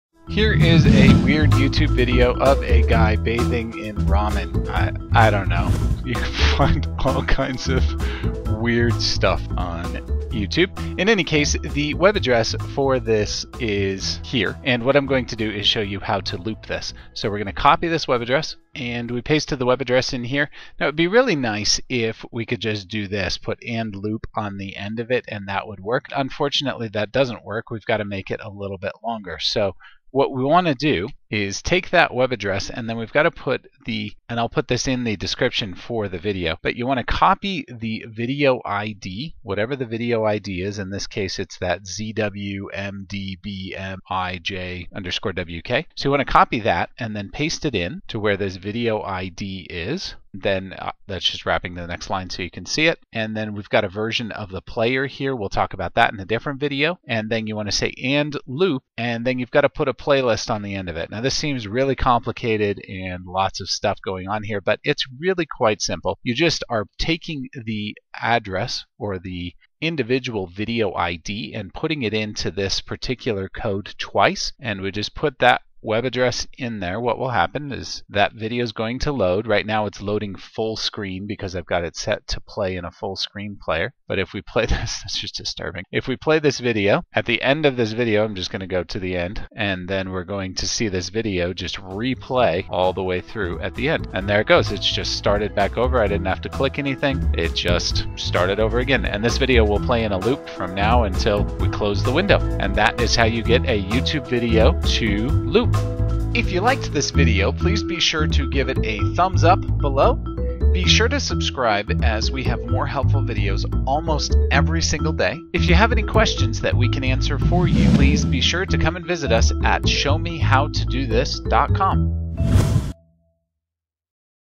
Looping: How To Get A YouTube Video to Loop
Looping a YouTube video so that it will play over and over and over and etc. is pretty simply, once you know how.  You can't just add &loop=1.  You have the full code for looping.  But once you do, the video will loop ad nauseum from now until you close your browser window.